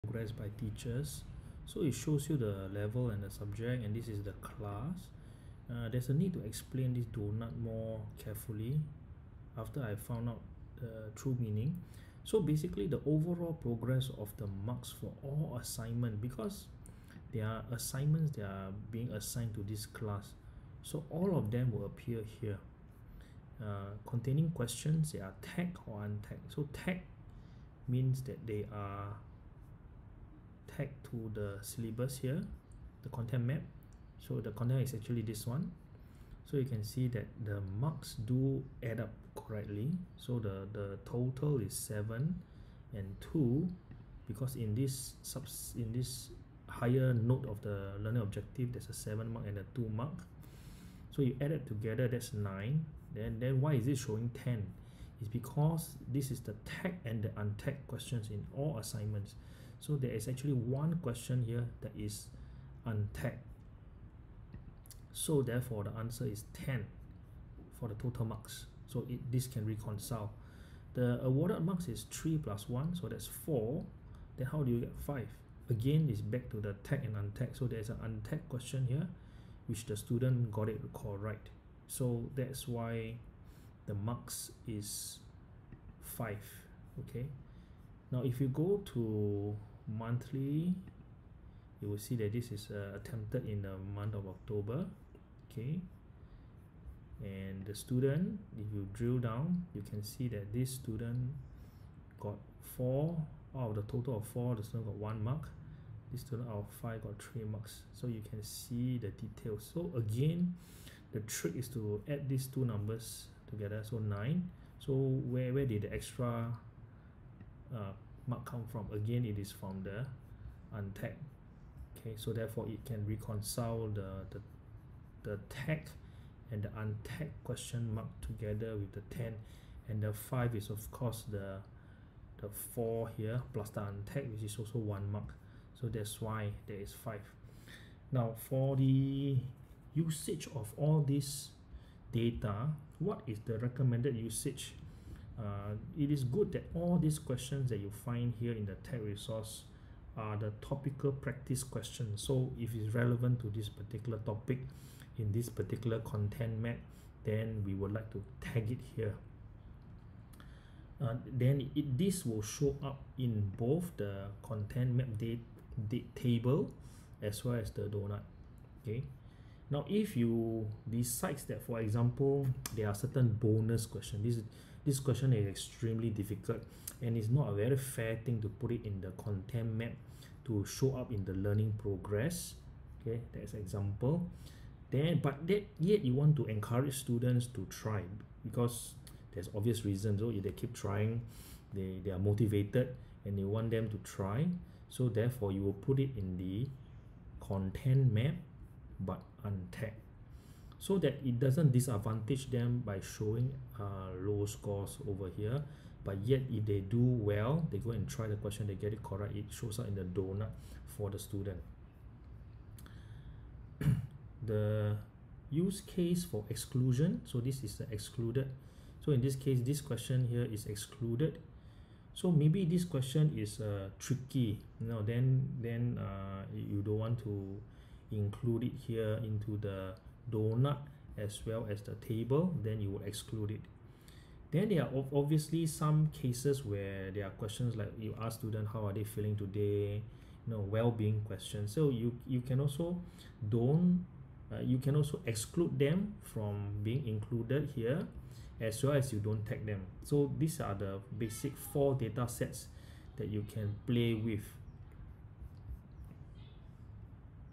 SLS R16 8a.1 Learning Progress part 8A.1.supplementary on data for Teachers, Live 8th Dec 2021
part 8a-1.supplementary on data types

This provides a consolidated view of the overall learning progress of students which includes marks of all teacher-assigned questions which are tagged to topics/subtopics in the chosen "Content Map", and “linked” to "Learning Progress".
Teachers should tag each question to only one subtopic instead of multiple subtopics to ensure accuracy in the "Learning Progress" data

Purpose
Enable teachers to monitor their class learning progress across different assignments by topic, subtopic, month and student.

FAQ
The donut is achieved marks available over total (tagged and untagged) marks possible included in the learning progress drawing marks from questions in assignment in SLS, not students achievement marks. On the SLS "learning progress" page, it states, "The overall progress includes the marks for all assignments containing questions (tagged or untagged to the current syllabus) that have been assigned to this class group." That is why there is the Excluded Questions will state  "Marks in questions that are excluded from Learning Progress will not be shown here. You may view or include these questions via “View Excluded Questions”.

SLS page states "Content map specific data can be viewed in the tabs below."
First tab of the table is total marks achieved, sort/group by topics, in content map by class
Second tab of table is achieved total marks by all students in class over maximum possible achievable marks, sort/group by month, in content map by class
Third tab is students achieved total marks over total possible sort/group by students, viewable by questions tagged, to see content topics, need to drill in and see the question tag.